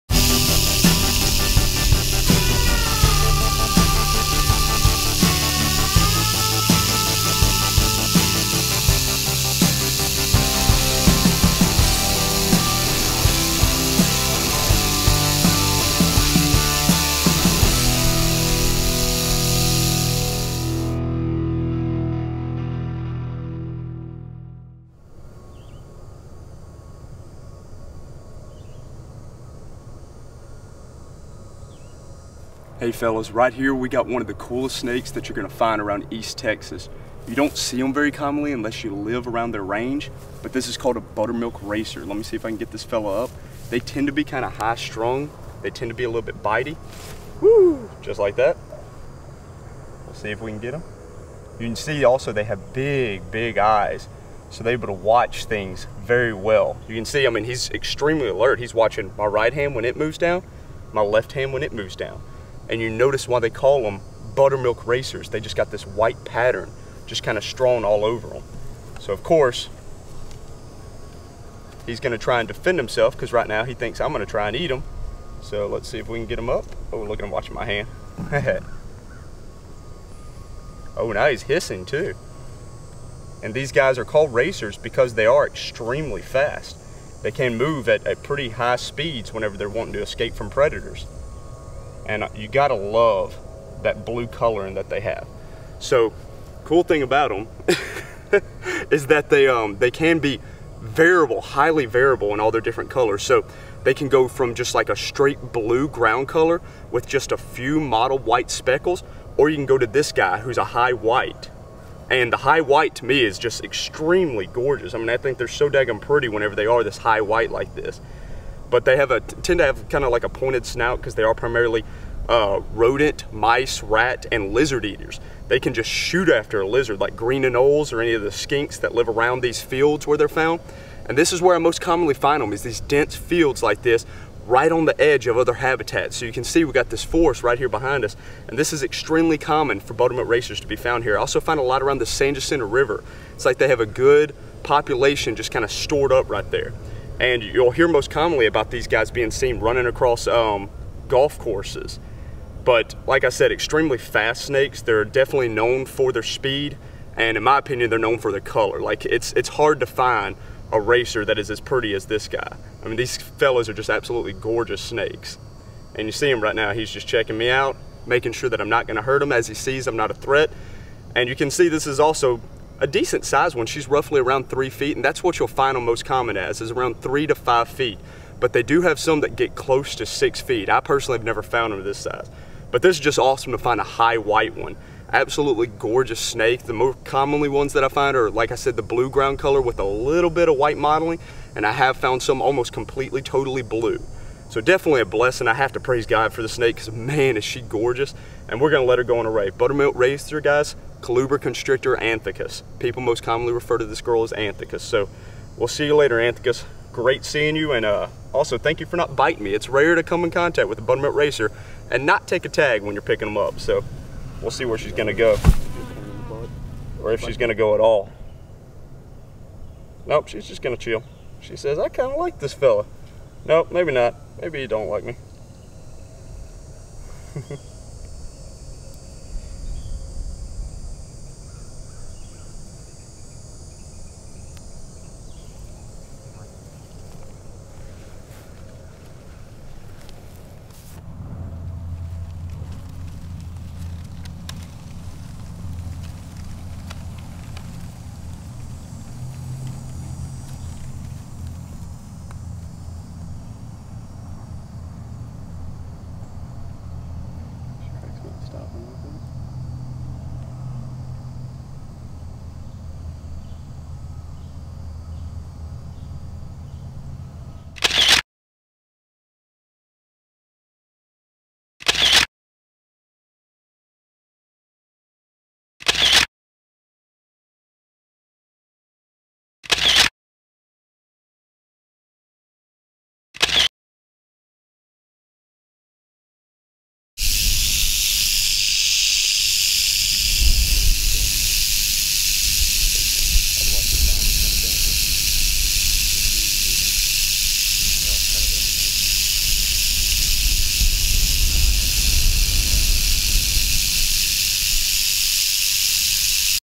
Buttermilk Racer
Hello, My name is Orry Martin: The Texas Snake Hunter.  This documentary is about the Buttermilk Racer (Coluber constrictor anthicus).  These beautiful, highly variable snakes are always a joy to see and this was my first high white one :)